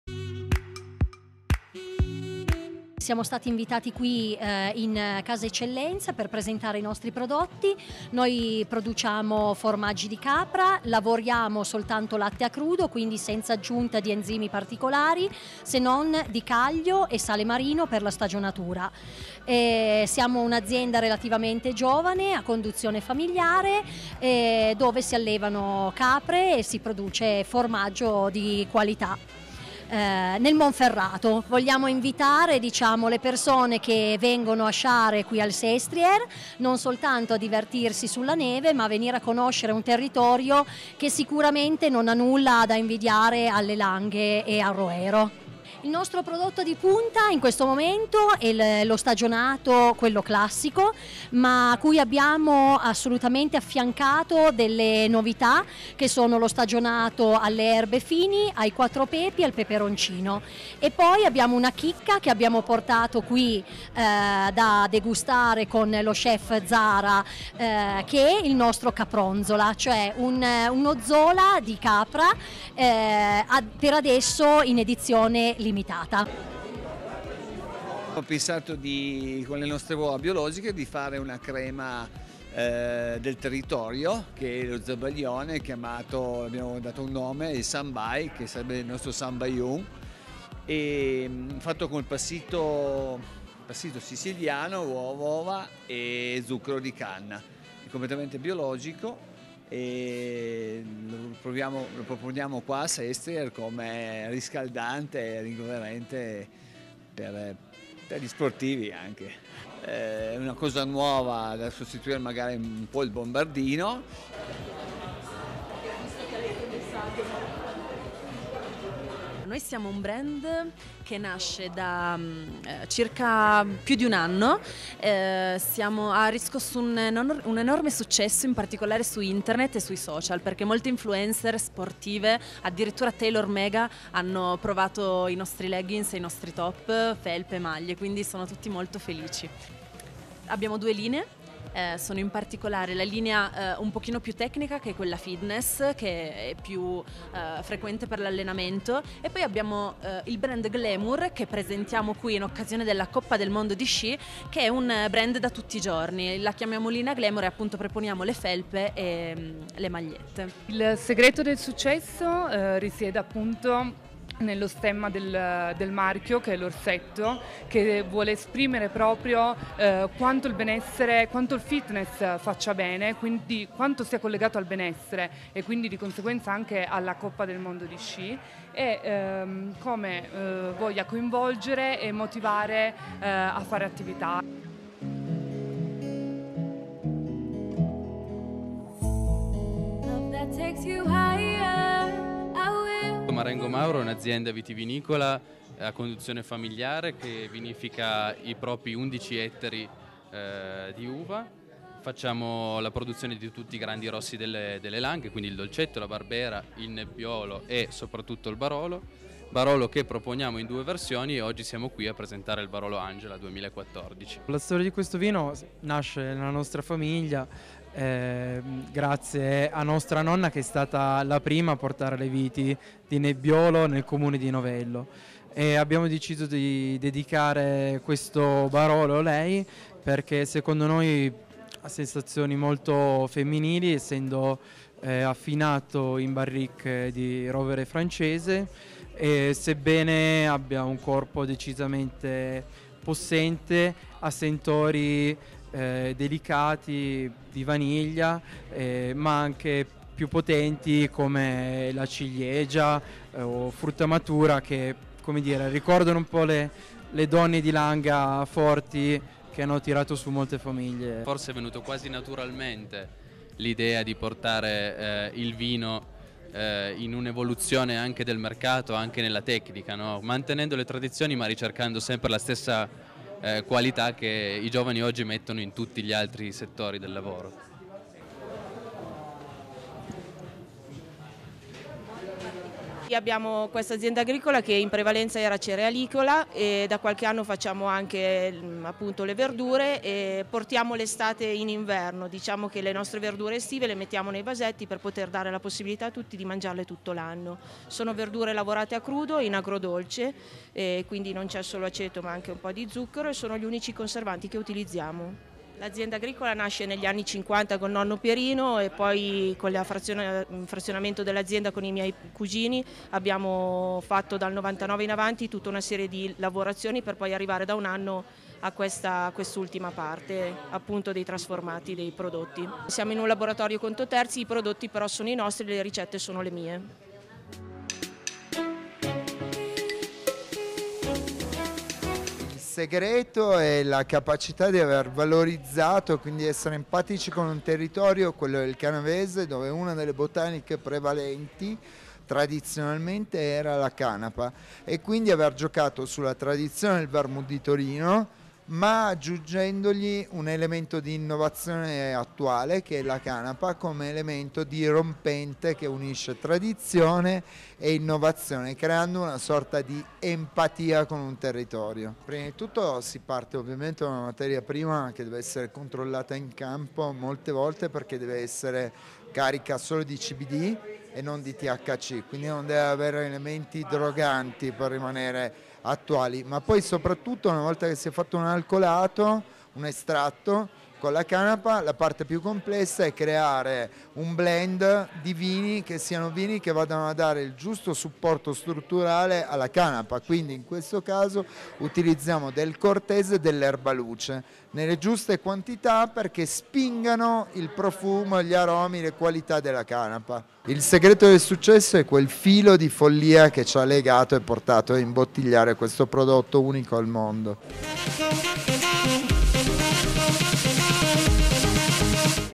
COPPA DEL MONDO SESTRIERE, DAL VERMOUTH AL BAROLO ALLA SCOPERTA DEL PIEMONTE
I produttori delle eccellenze artigiane ed enogastronomiche del Piemonte protagonisti a 'Casa Eccellenze' lo spazio dove il pubblico e gli atleti della Coppa del Mondo di sci a Sestriere può scoprire e degustare i prodotti anche grazie agli showcooking degli chef stellati.